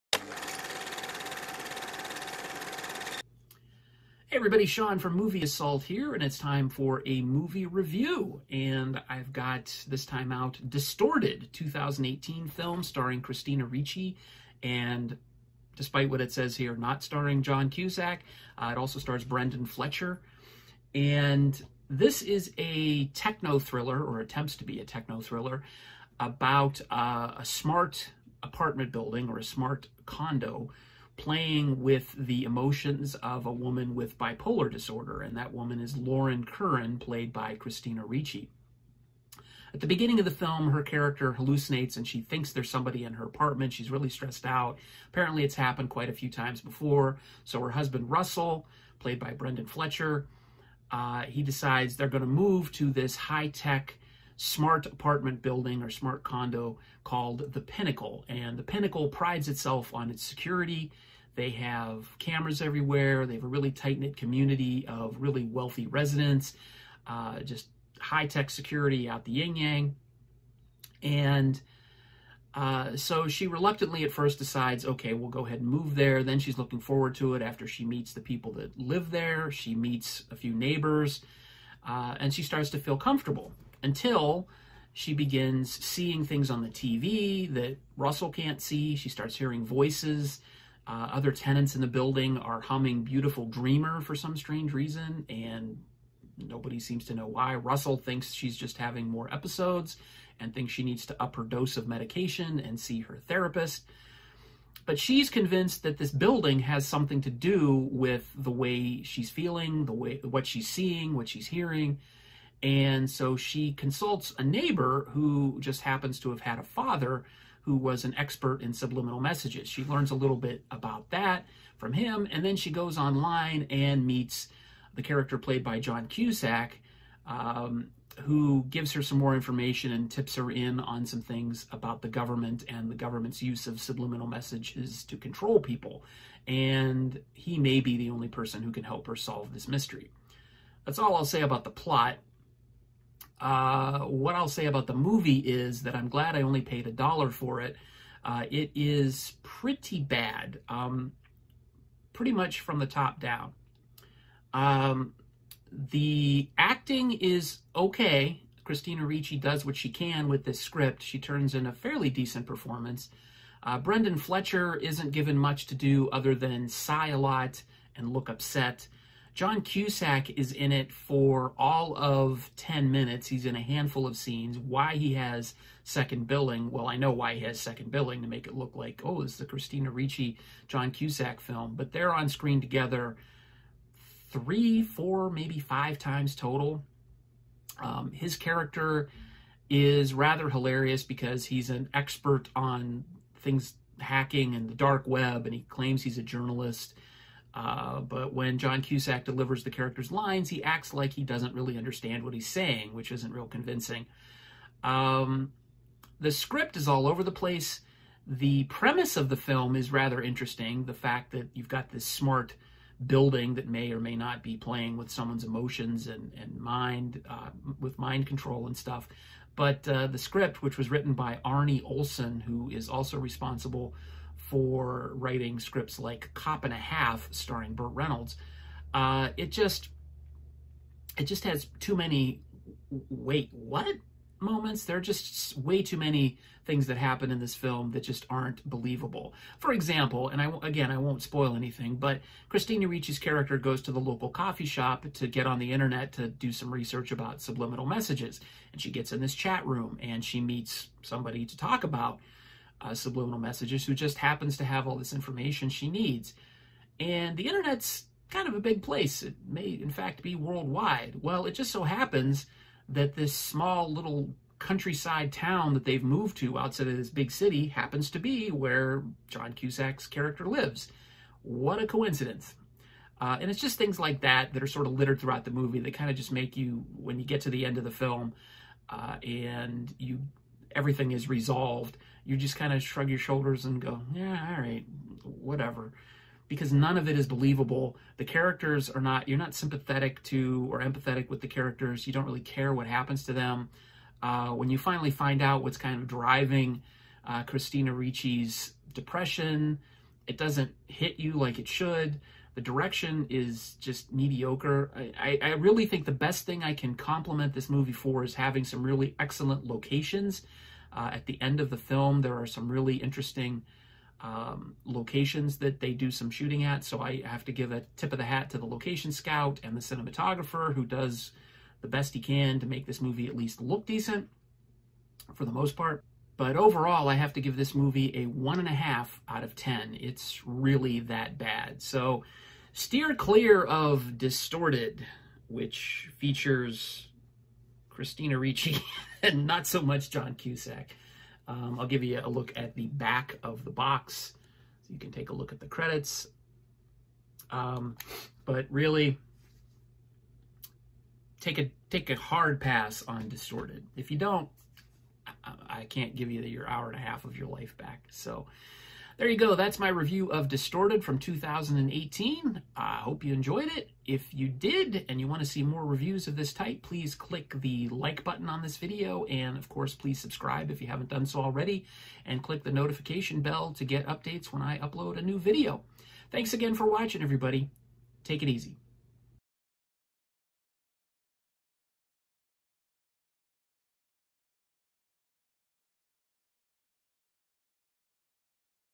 Distorted (2018) Review
A review of the 2018 techno-thriller, Distorted, starring Christina Ricci, Brendan Fletcher and featuring John Cusack. The film was directed by Rob W. King and written by Arne Olsen.